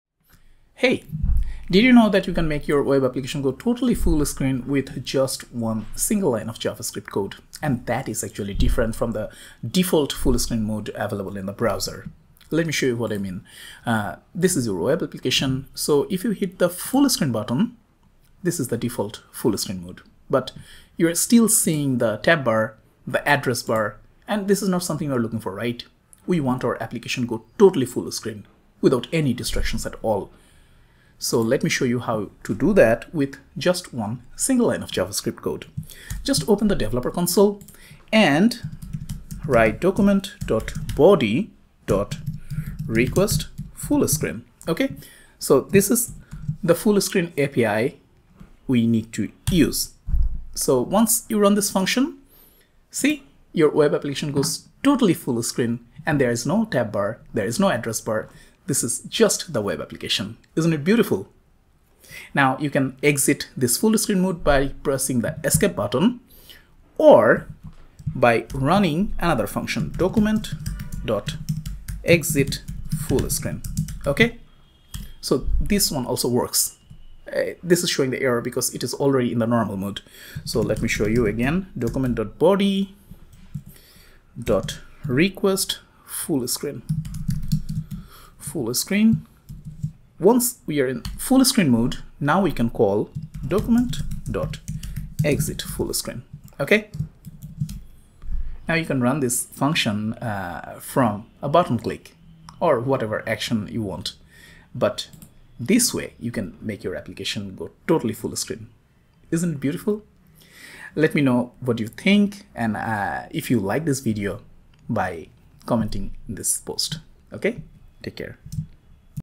FullScreen Mode with a single line of JavaScript Code
Show your application in fullscreen mode with this single line of javaScript code.